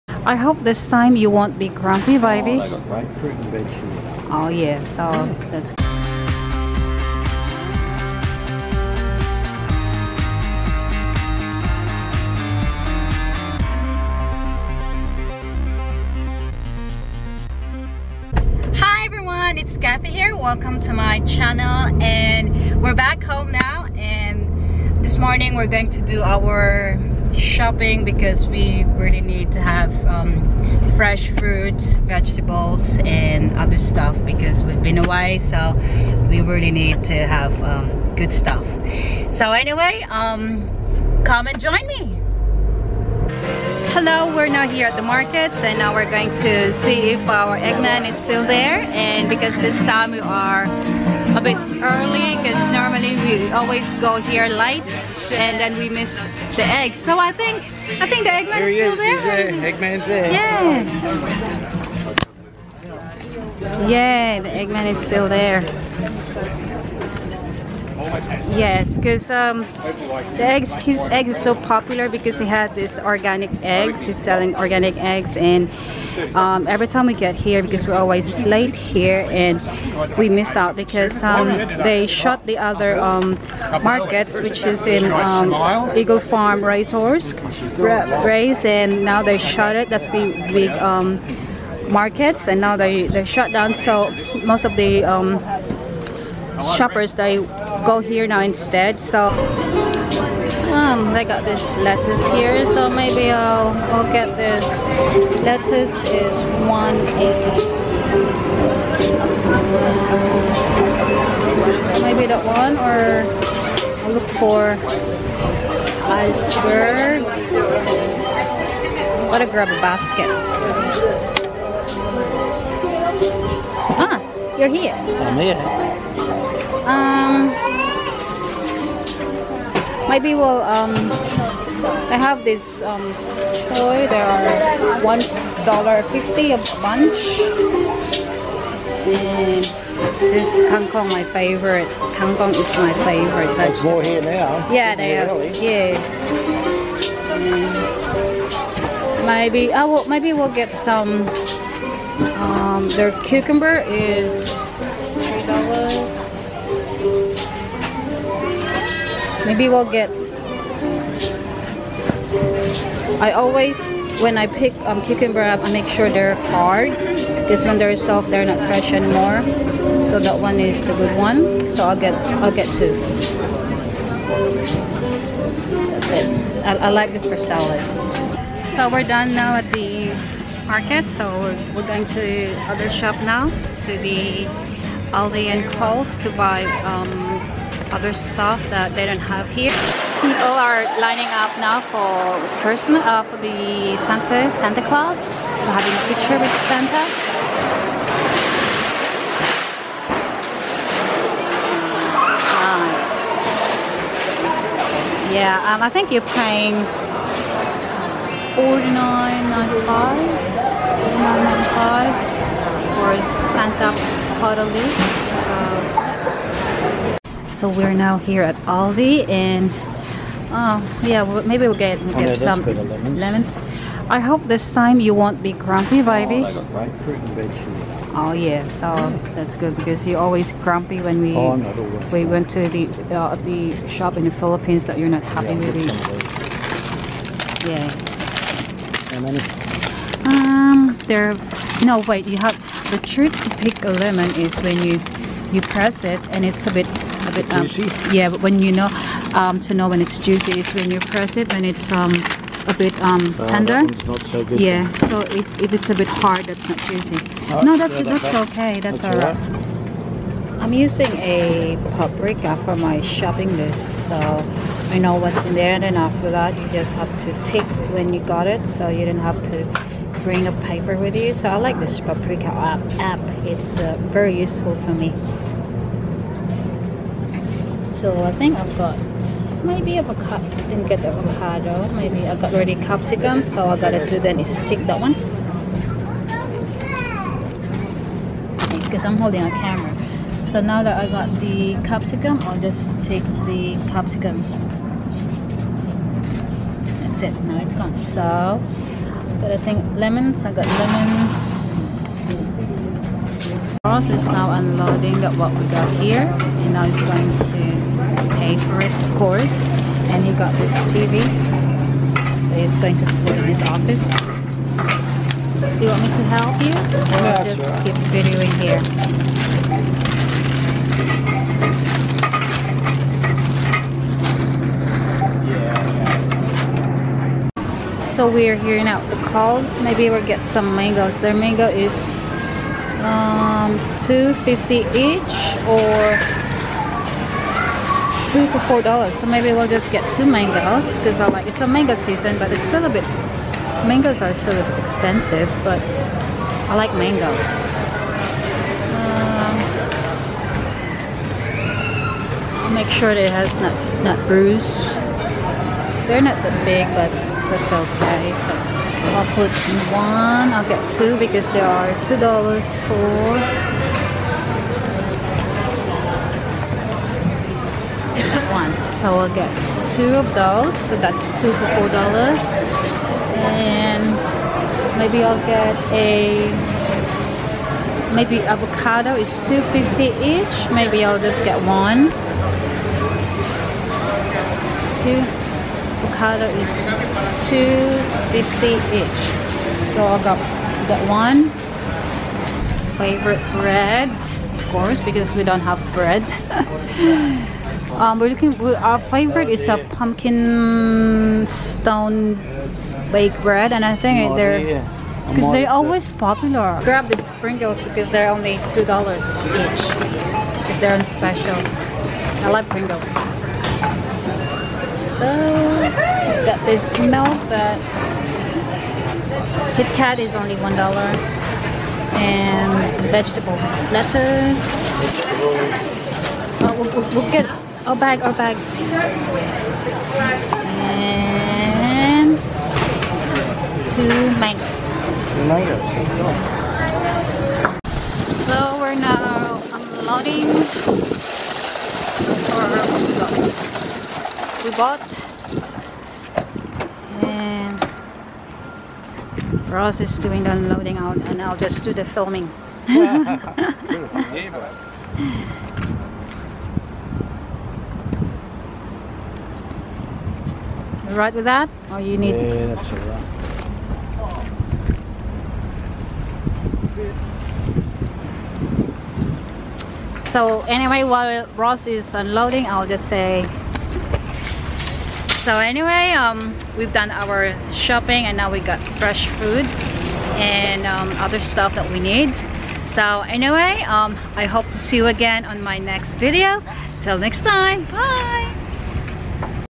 FILIPINA WIFE IN AUSTRALIA | SHOPPING FOR VEGETABLES, FRUITS AND BREAD
Filipina wife and her Australian husband have their shopping for vegetables and fruits, to restock the fridge after a few weeks holiday to the Philippines. They went to the local farmers market to buy vegetables and fruits, and to Aldi and Coles for other groceries that are not available at the market.